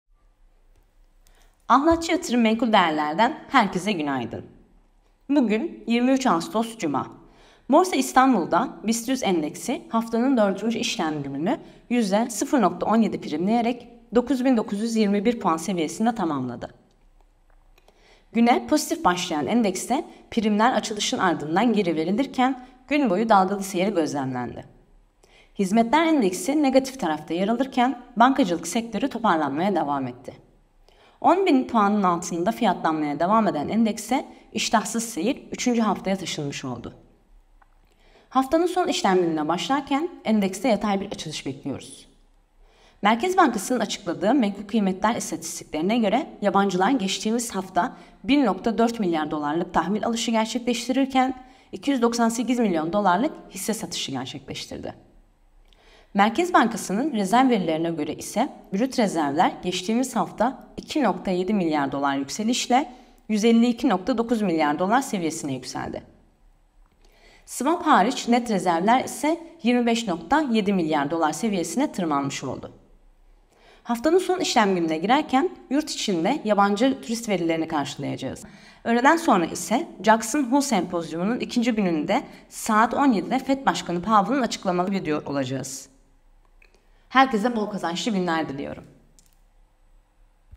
YatırımGünlüğü Piyasalar ve Beklentiler - 23 Ağustos - Cemile Gültekin
Gün içi piyasaları ve beklentileri değerlendirdiğimiz Yatırım Günlüğü her gün sizlerle Ahlatcı Yatırım YouTube'da!

➡️Sosyal medyada bizi takip ederek günlük gelişmelerden haberdar olun.